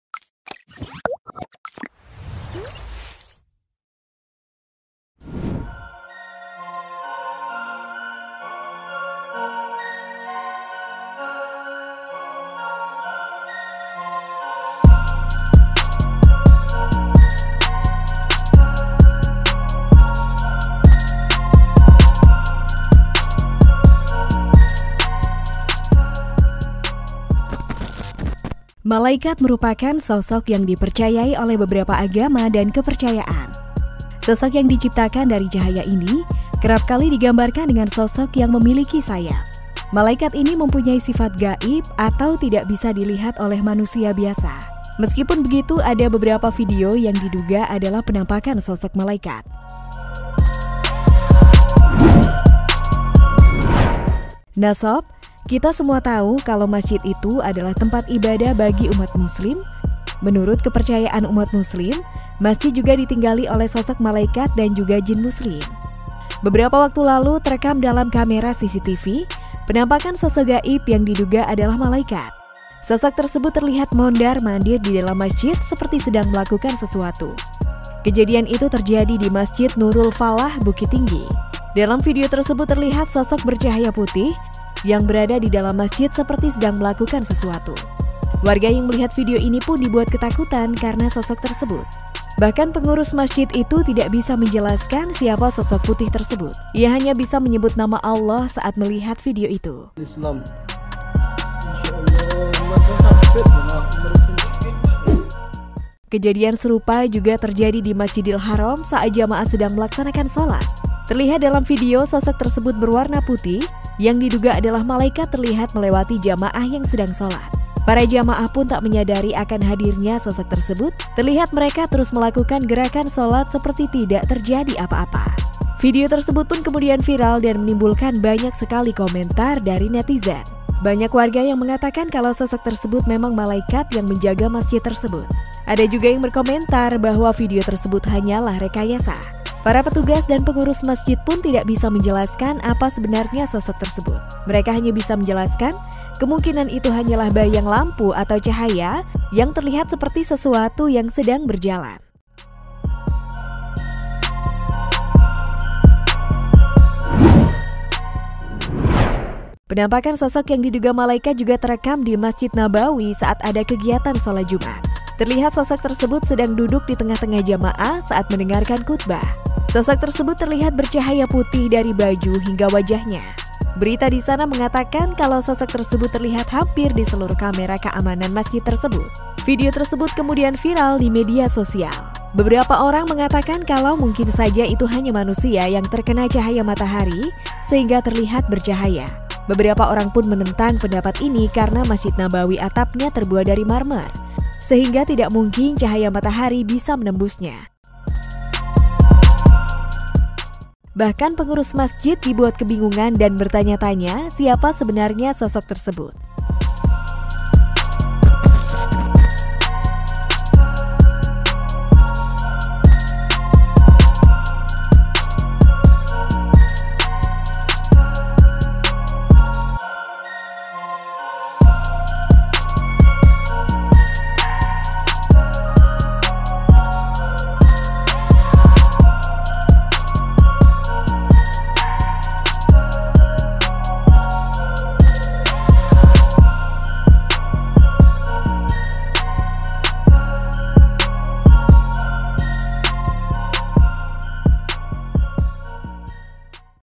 SUBHANALLAH! Saat Khotbah Jumat, Sosok Bercahaya Tiba2 Muncul Di Masjid | Penampakan Malaikat
Malaikat merupakan sosok yang di percayai oleh beberapa agama dan kepercayaan. Sosok yang di ciptakan dari cahaya ini kerap kali di gambarkan dengan sosok yang memeliki sayap. Malikat ini mempunyai sifat ghaib atau tidak bisa di lihat oleh manusia biasa. Meskipun begitu ada beberepa video yang diduga adalah penampakan sosok maliakat.